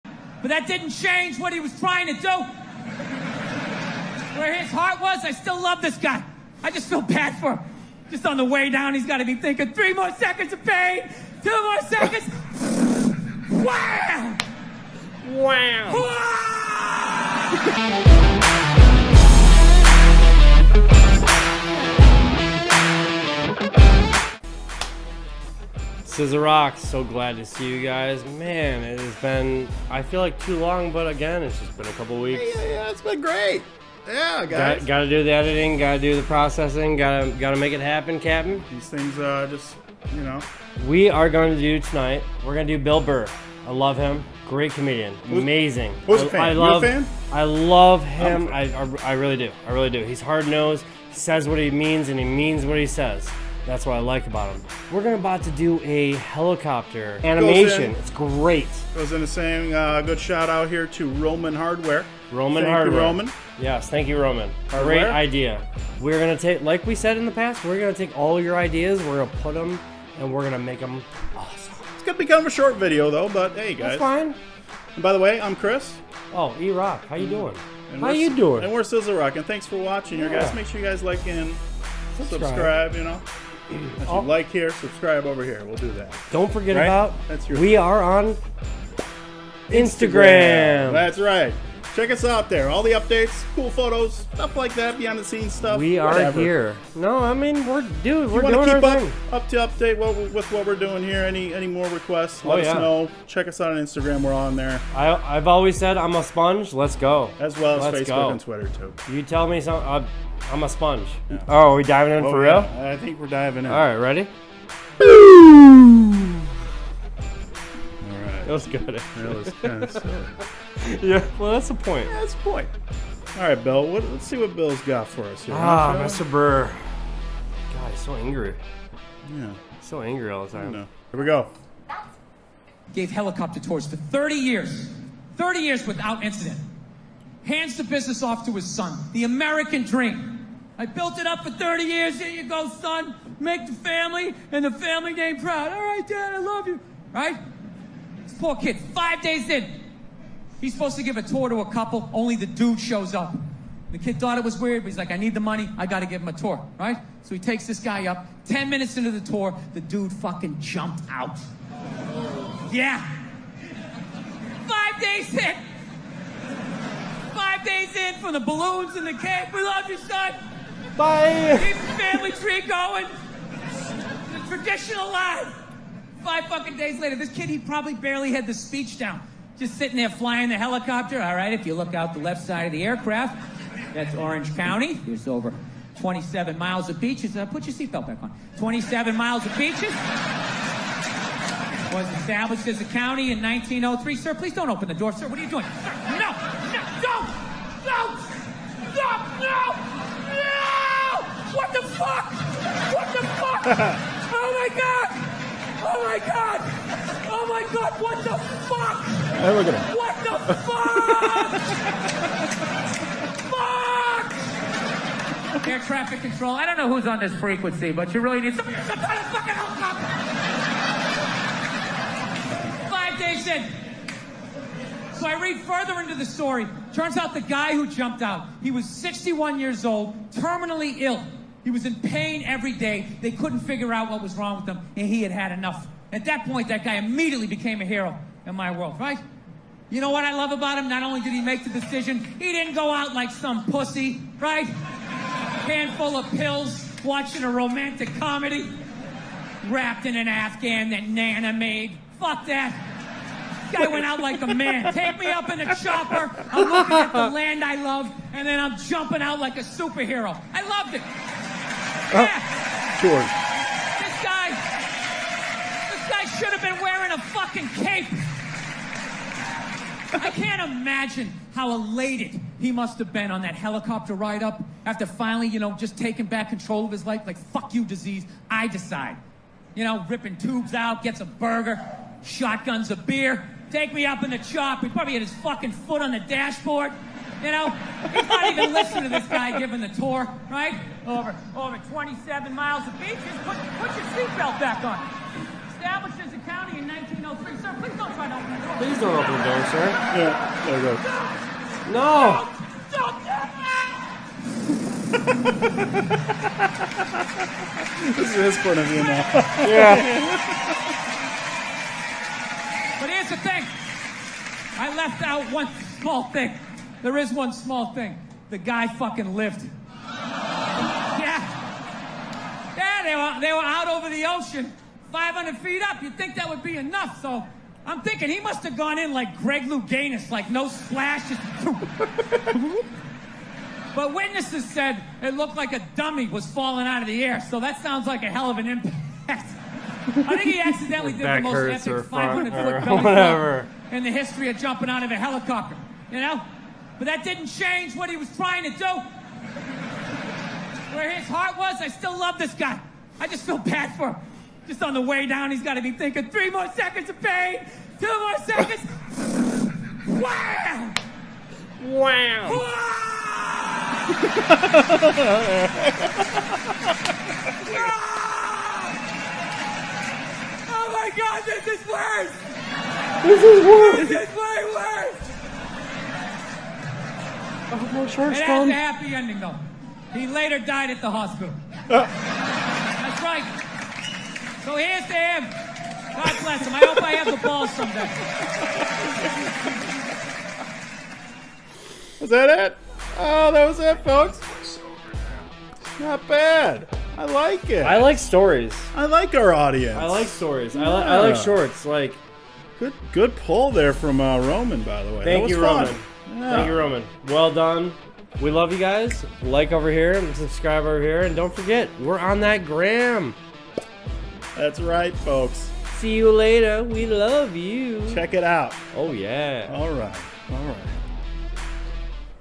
BILL BURR'S HELICOPTER STORY! | REACTION VIDEO | Sizzle Rock
Watch Bill Burr's Helicopter Story With Sizzle Rock! It's Time For Another Reaction Video!

New to the channel? Like what you see? Join us on our journey!

-Sizzle Rock